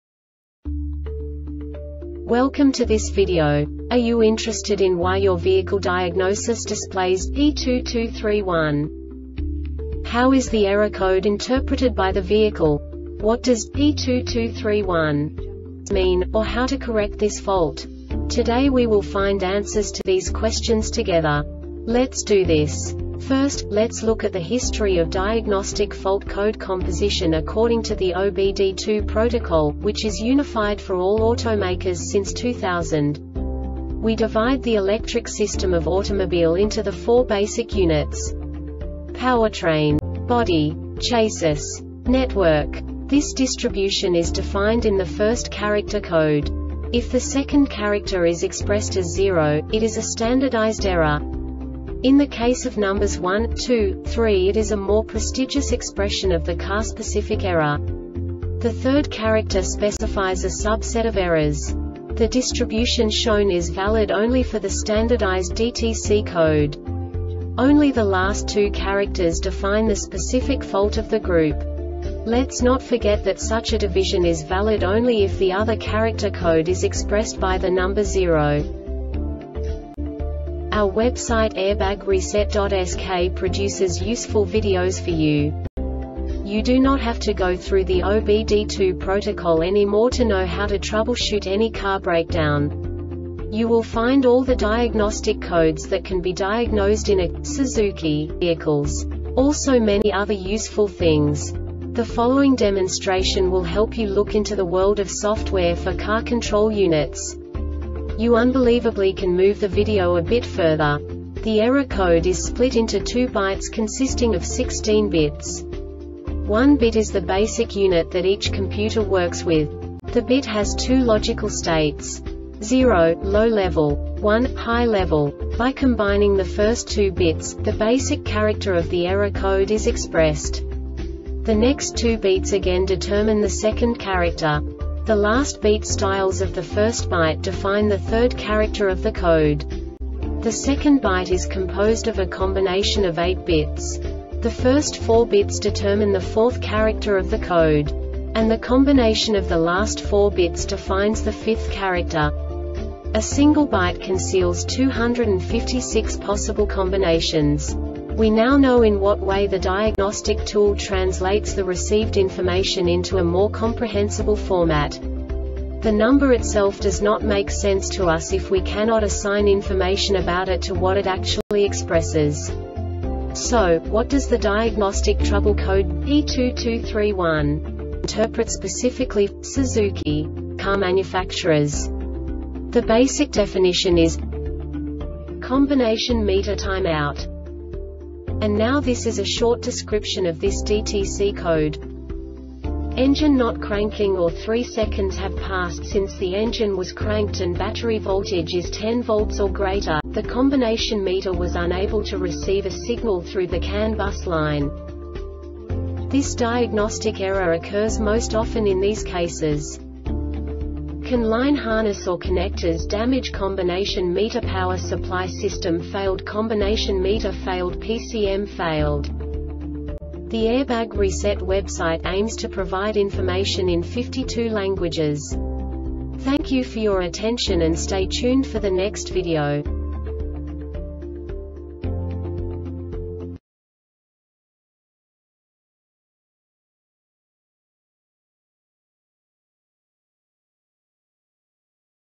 DTC Suzuki P2231 Short Explanation
Contents:
0:21 Basic DTC analysis according to OBD2 protocol standard.
1:48 Insight into programming
2:58 A diagnostically understandable description of the Diagnostic Trouble Code
3:05 Basic error definition

Make:
Suzuki
Code:
P2231
Definition:
Combination Meter Timeout
Description:
Engine not cranking or 3 seconds have passed since the engine was cranked and battery voltage is 10 volts or greater,the combination meter was unable to receive a signal through the CAN bus line.
Cause:
CAN line harness or connectors damageCombination meter power supply system failedCombination meter failedPCM failed